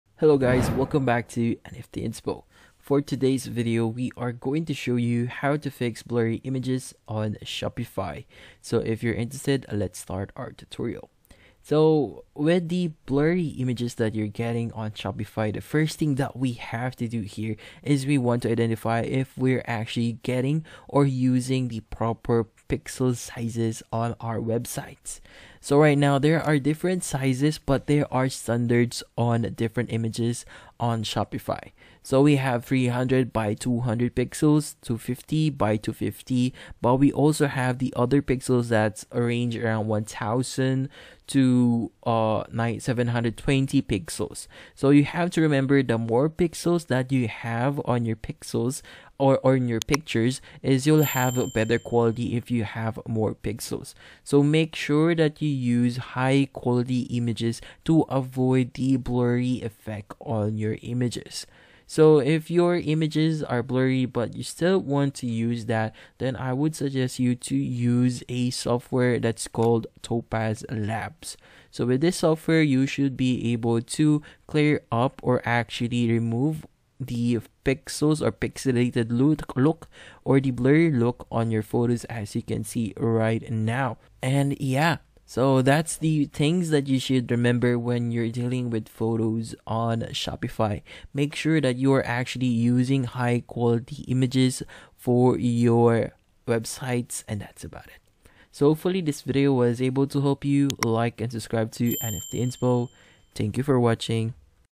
How To Fix Blurry Images On Shopify (2025)
If you want to fix blurry images on shopify then this video will be perfect for you!